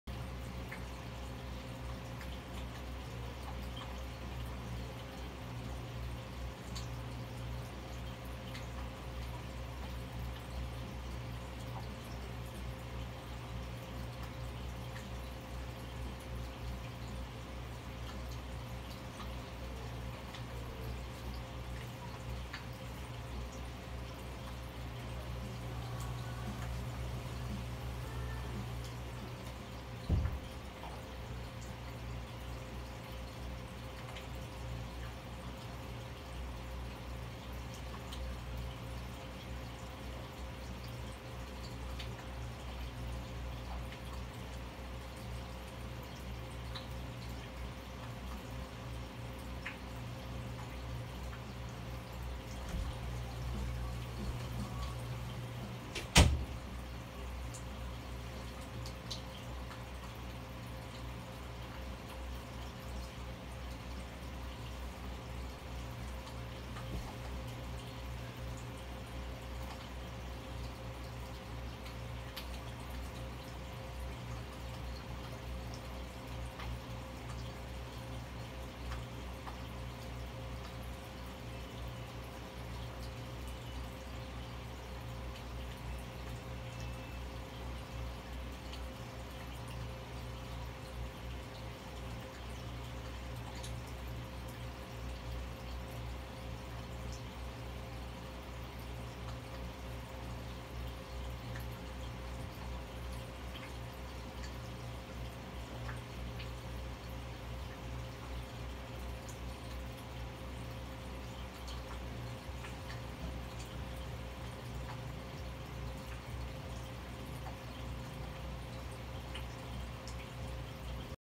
My New Fish
I just got these guys a few hours ago. There are more fish see if you can see them in the back Round